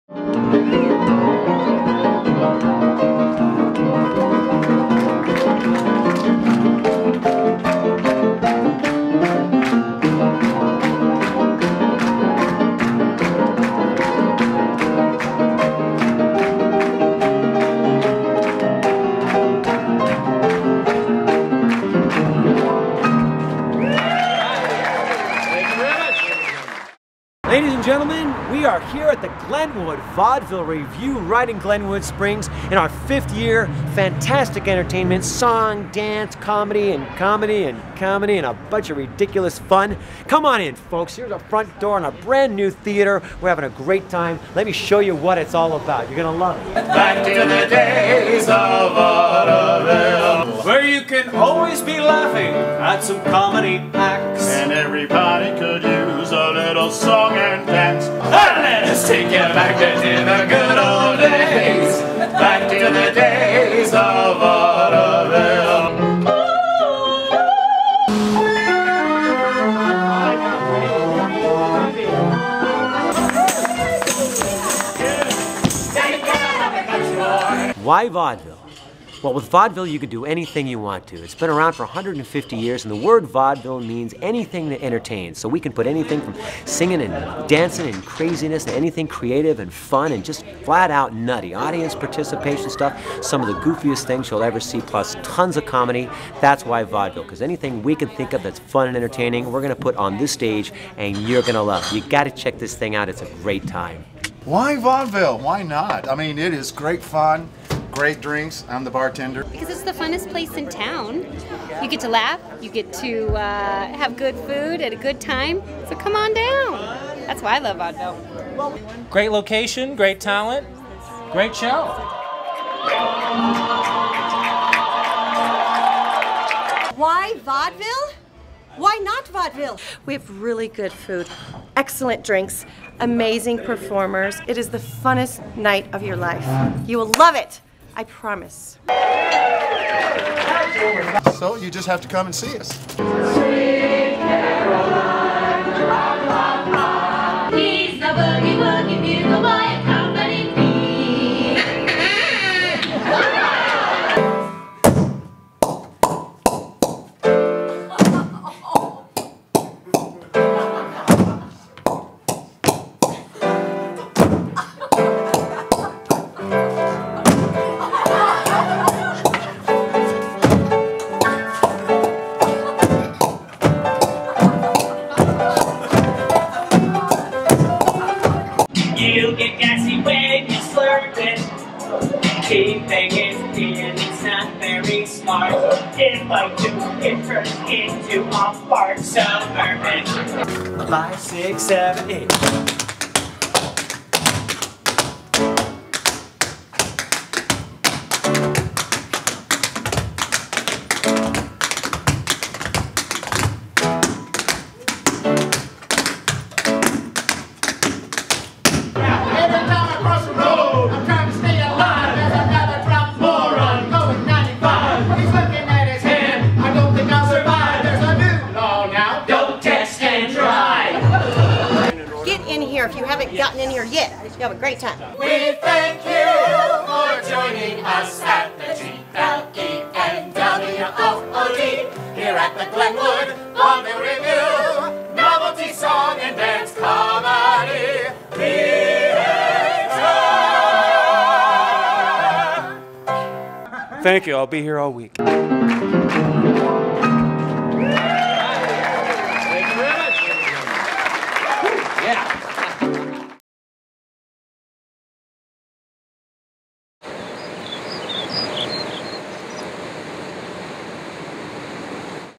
2014 Glenwood Vaudeville Revue
The Glenwood Vaudeville Revue is a two hour family fun dinner theater show with professional talent performing a variety of comedy skits, jokes, high energy dance numbers, unique novelty songs and original comedic presentations that are guaranteed to entertain, and help you laugh your worries away; and all this while you savor a meal prepared by many of the Glenwood Springs best restaurants.